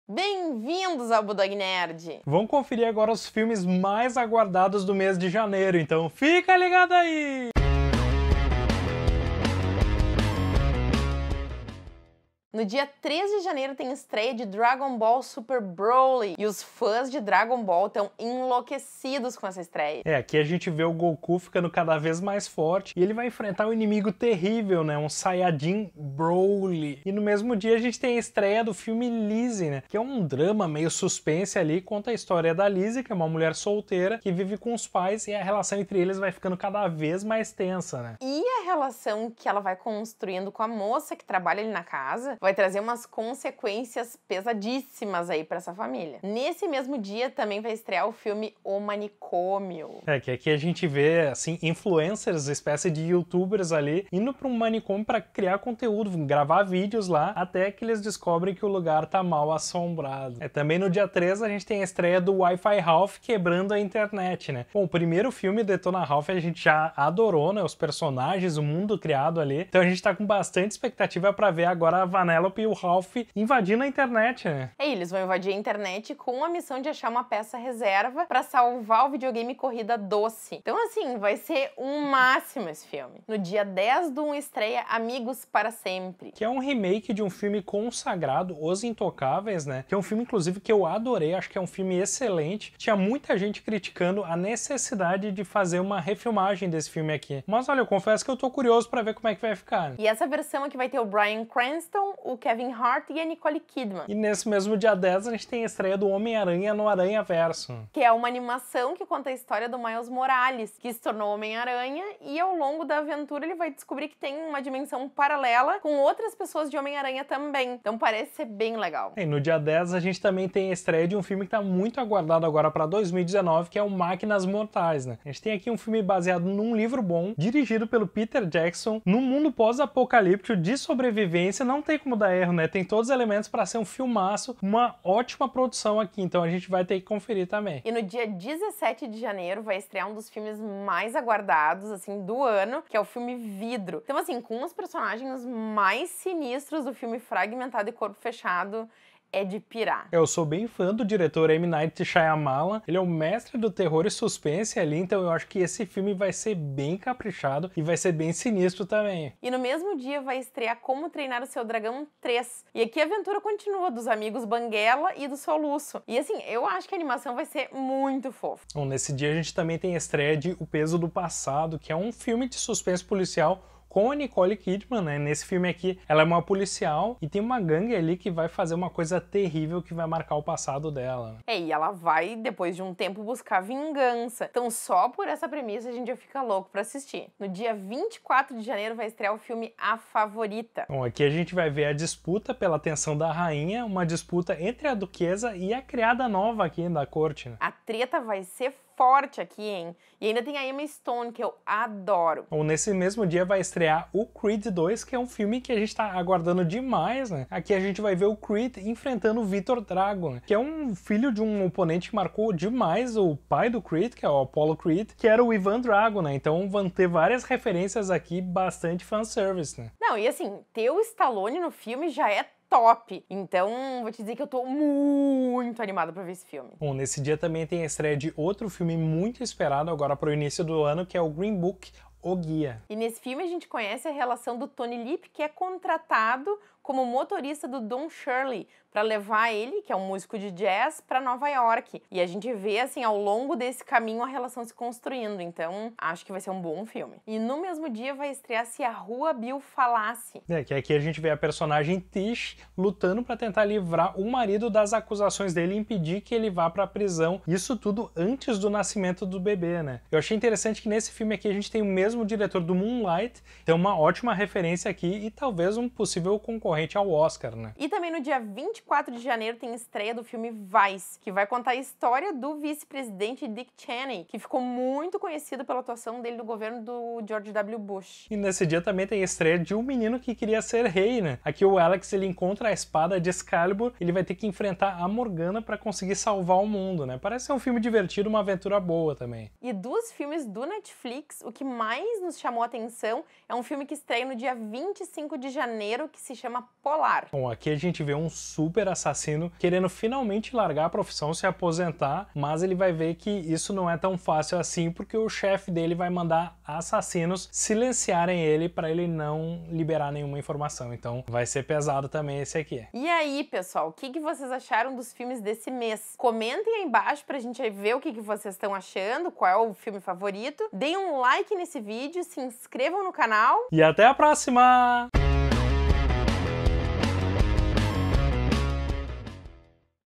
FILMES de JANEIRO - 2019
Principais estreias de filmes do mês de janeiro de 2019.

Não percam nossas LIVES, que acontecem sempre seg-ter-qui-sex às 19:30 no Facebook